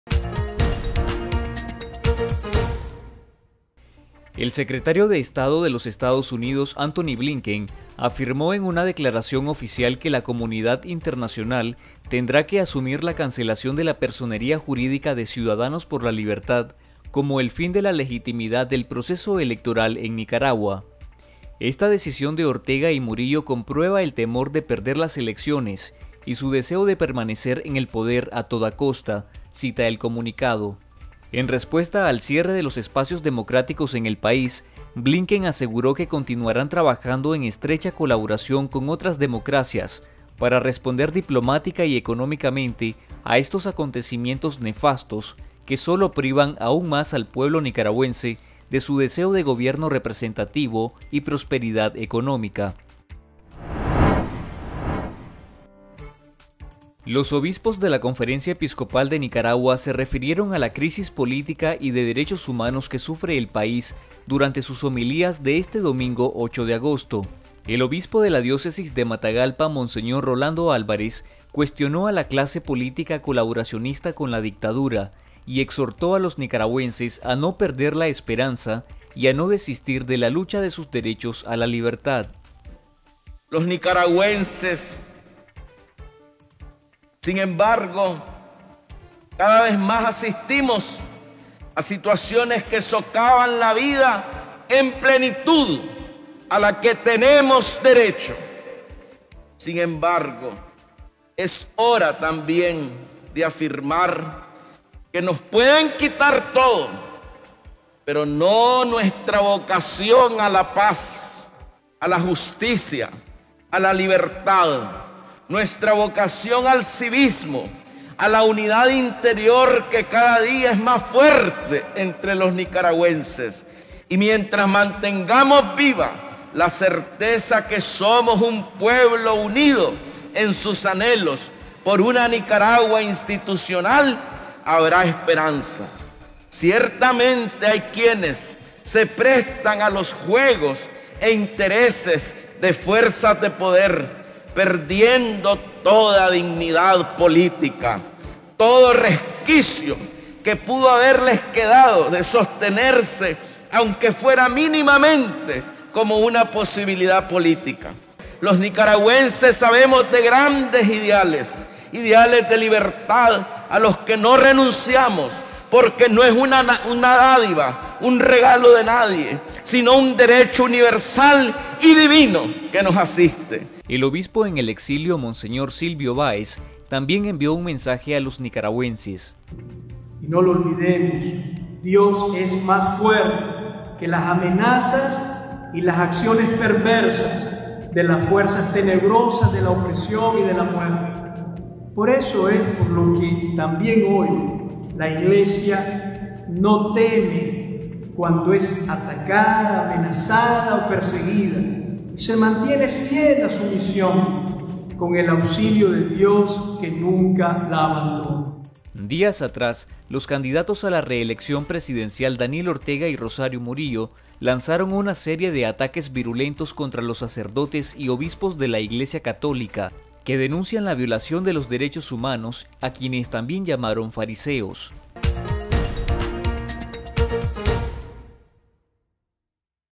Antony Blinken: “Golpe final a elecciones libres y justas en Nicaragua”
El Secretario de Estado de los Estados Unidos, Antony Blinken, emitió una declaración la tarde de este sábado afirmando que la comunidad internacional tendrá que asumir que la cancelación de la personería jurídica de Ciudadanos por la Libertad (CxL) por parte del Consejo Supremo Electoral (CSE), significaba el fin de la legitimidad del proceso electoral de Nicaragua.

“La decisión del presidente Daniel Ortega y la vicepresidenta Rosario Murillo este 6 de agosto de prohibir al último partido genuino de oposición participar en las elecciones de noviembre, subraya su deseo de permanecer en el poder a toda costa. Esta maniobra autocrática sigue a la detención de siete candidatos presidenciales y otras 24 figuras de la oposición, activistas de derechos humanos, líderes empresariales, estudiantes y trabajadores de ONG durante los últimos dos meses. Estados Unidos considera las últimas acciones autoritarias y antidemocráticas del régimen, impulsadas por el temor de Ortega a perder las elecciones, como el golpe final contra las perspectivas de Nicaragua de unas elecciones libres y justas a finales de este año. Ese proceso electoral, incluidos sus eventuales resultados, ha perdido toda credibilidad”, cita el comunicado del Secretario de Estado.